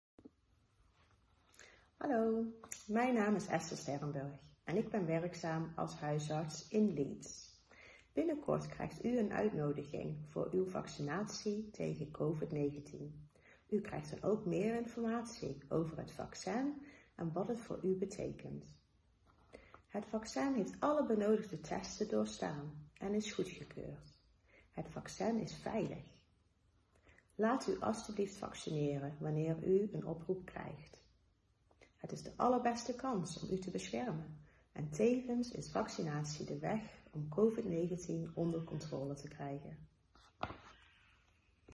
Receiving your COVID-19 vaccination - Dutch (short version)
Dr Esther Sterrunberg explains in Dutch what to expect from your coronavirus vaccination appointment after being invited by the NHS.